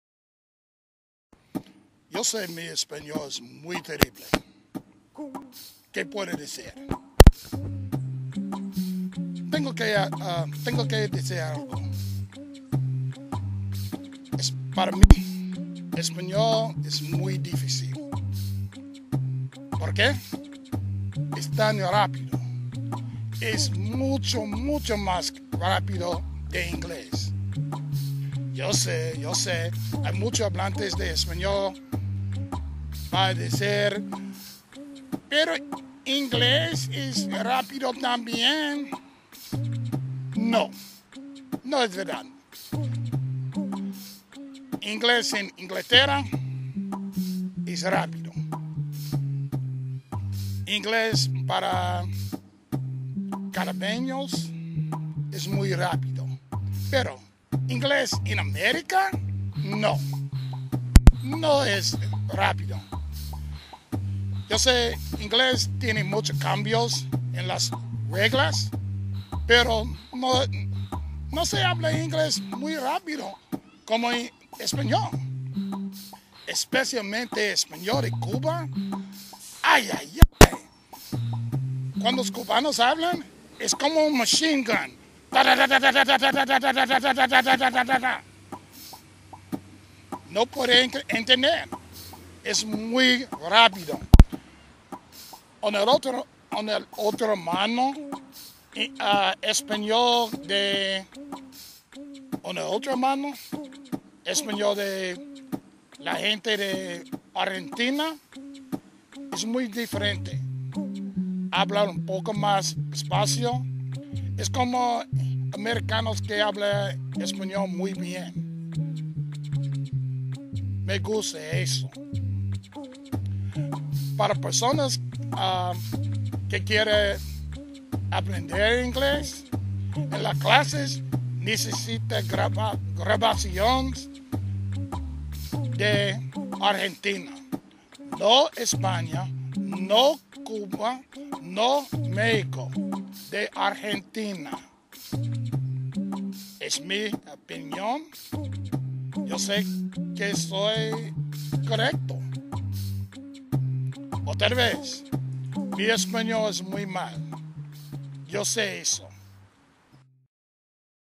ESPANOL ES TAN RAPIDO
Puedo escribir español y puedo leer español, pero no puedo hablar y escucharlo bien. Es tan rapido... mucho mas que ingles.